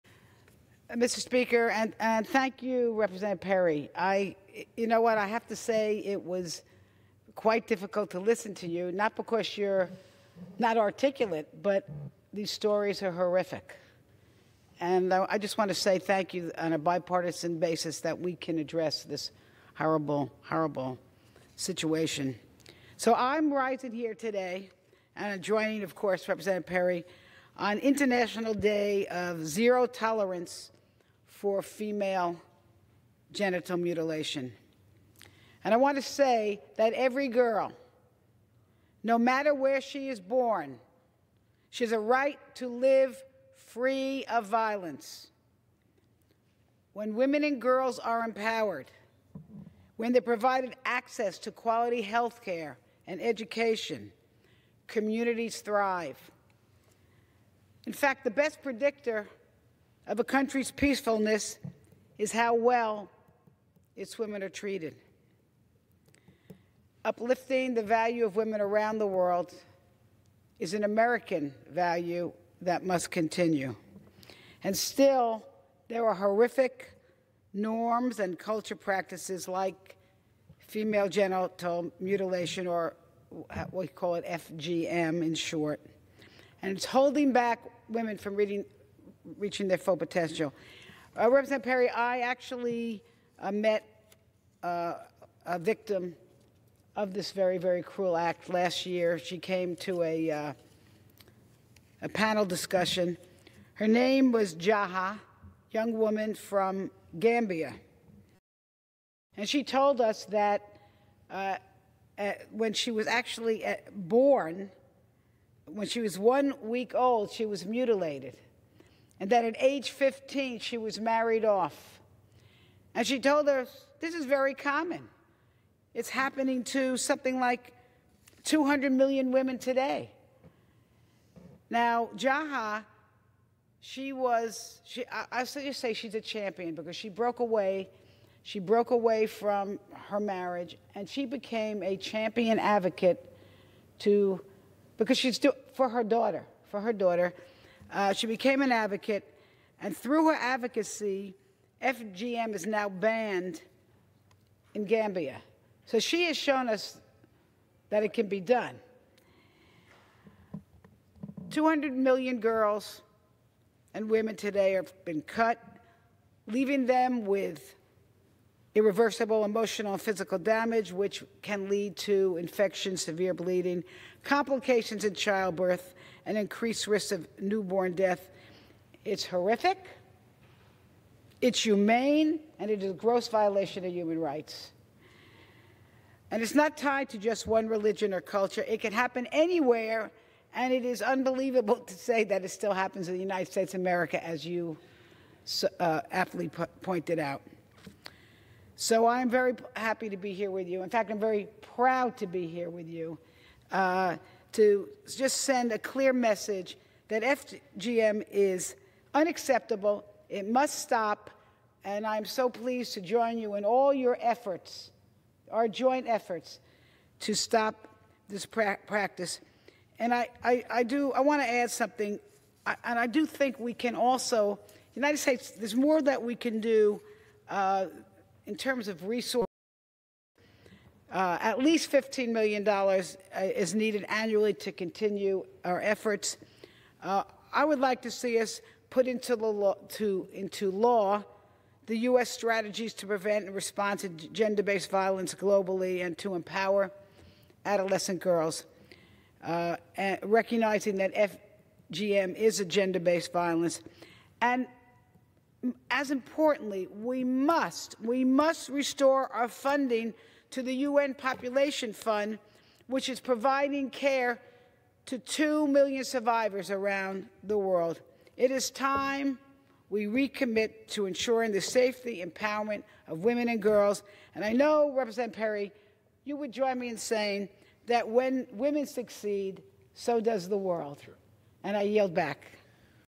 Frankel Denounces Female Genital Mutilation as a Violation of Human Rights
On the International Day of Zero Tolerance for Female Genital Mutilation, Congresswoman Lois Frankel and Congressman Scott Perry introduced a resolution to enhance global awareness of female genital mutilation/cutting (FGM/C), recognize it as a human rights violation against women and girls, and urge the U.S. Government and international community to undertake coordinated efforts to eliminate the harmful practice.